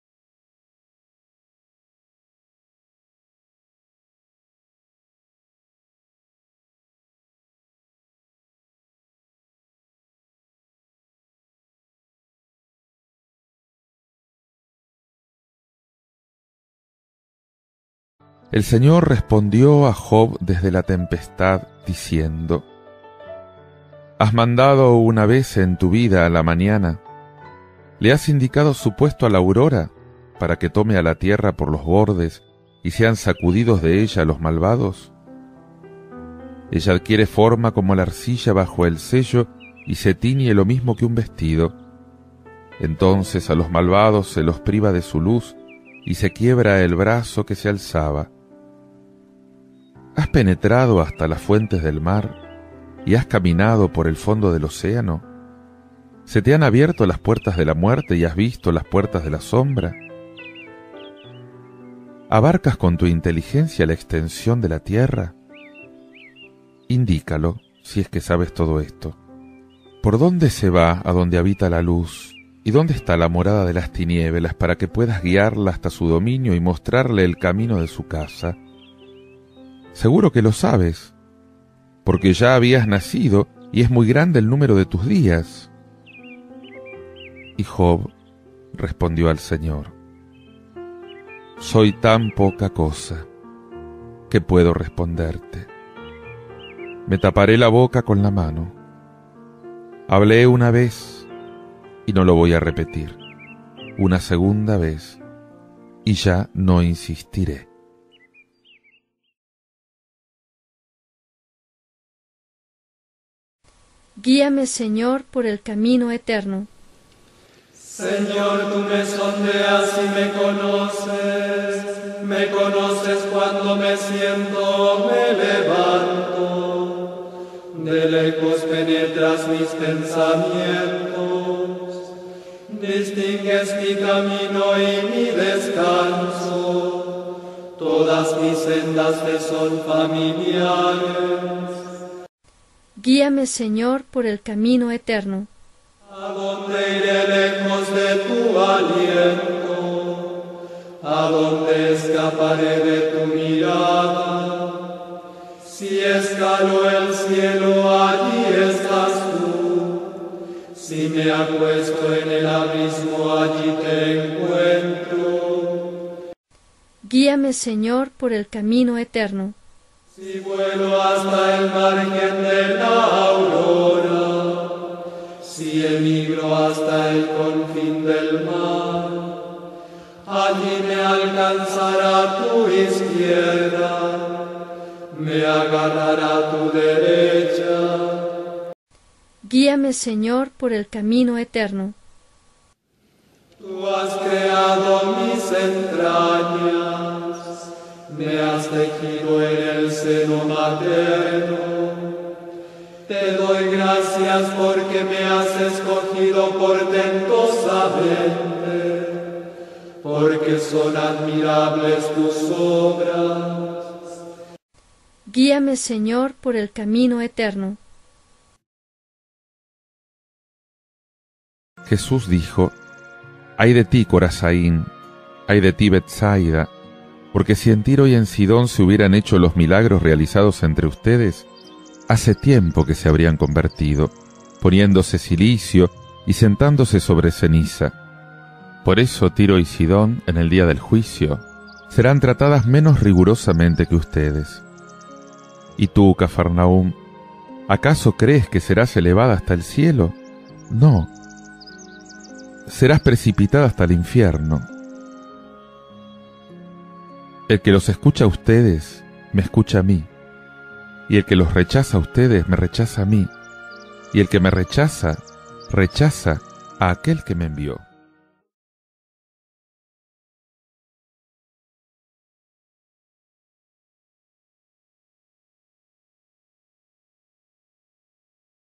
Evangelio del día, Lecturas Viernes 30 de Septiembre de 2016 XXVI Semana Tiempo Ordinario
Evangelio del día, lecturas Viernes 30 de Septiembre de 2016 XXVI SEMANA DEL TIEMPO ORDINARIO CICLO C. PRIMERA LECTURA: Del libro de libro de Job 38,1.12-21;40,3-5 SALMO: 138 R/. Guíame, Señor, por el camino eterno EVANGELIO: Del Santo Evangelio según San Lucas 10,13-16